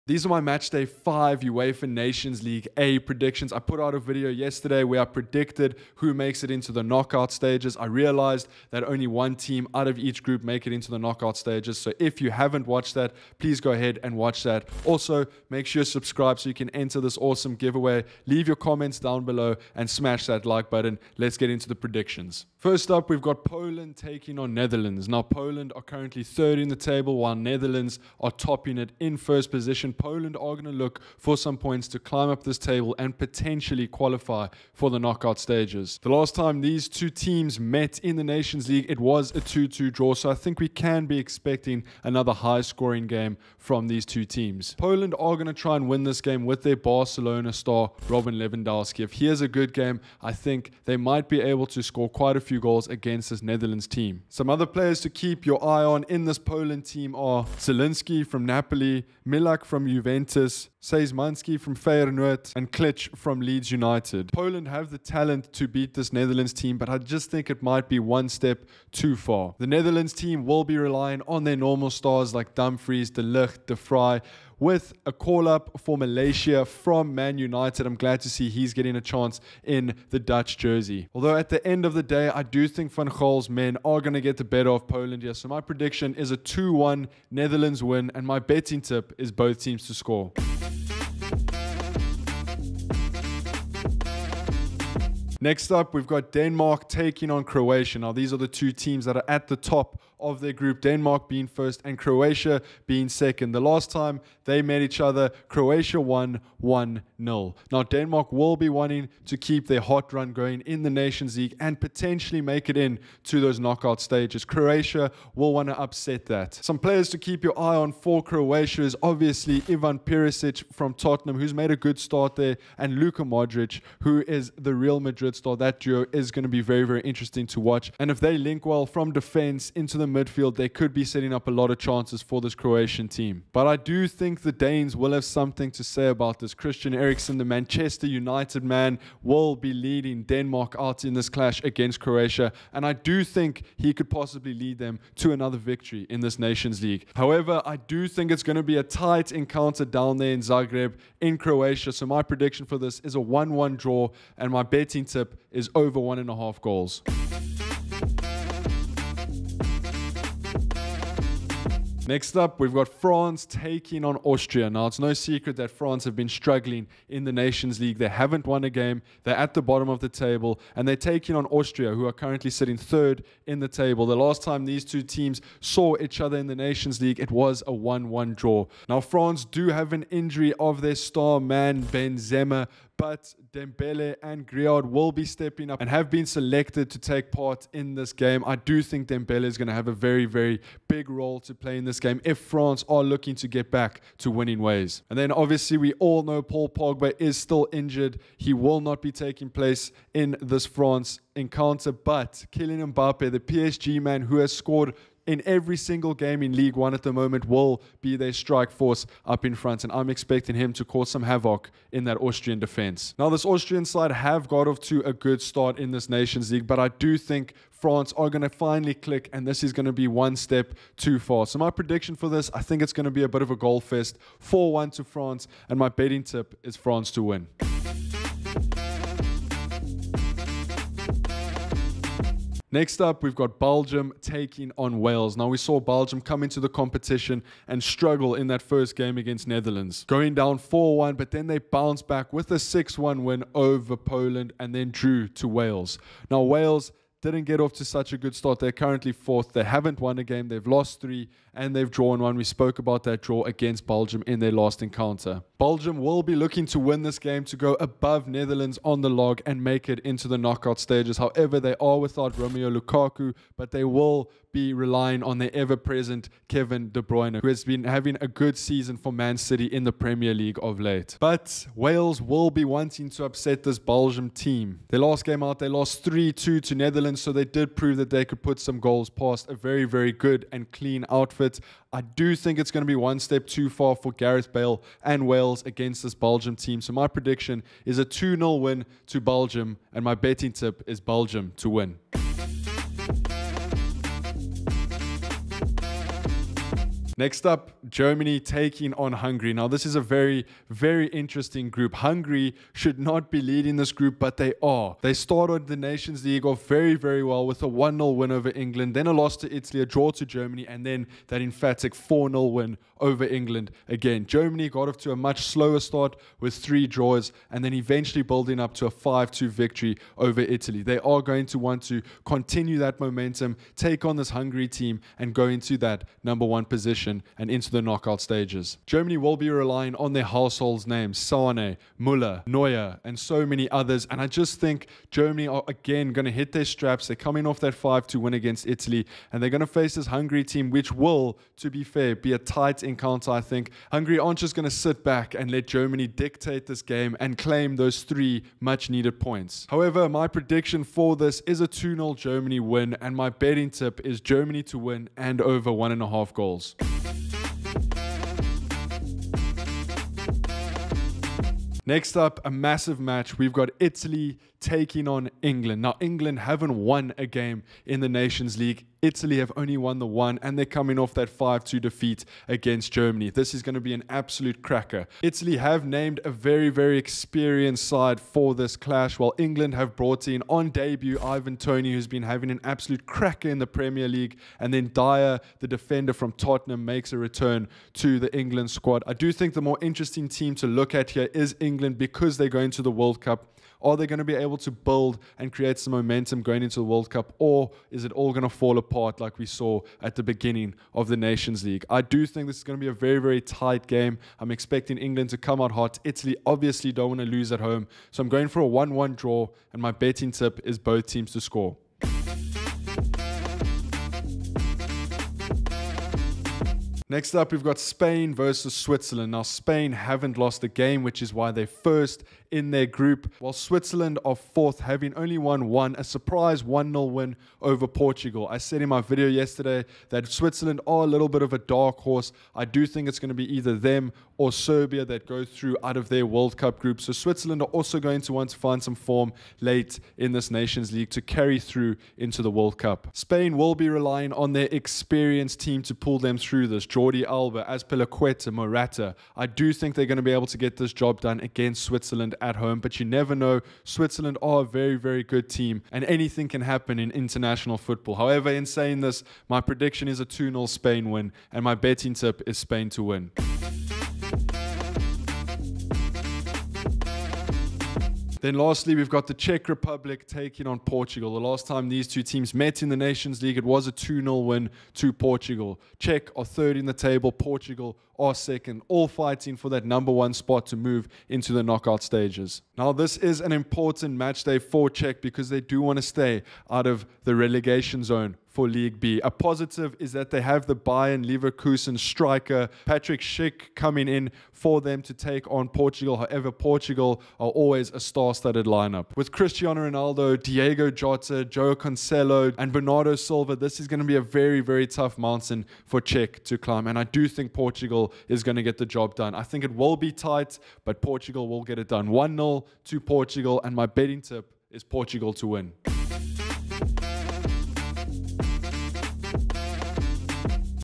Matchday 5 UEFA Nations League A Predictions
The Nations League is back! This is going to give us a good insight into what we can potentially expect from these teams in the World Cup. Leave your predictions in the comments!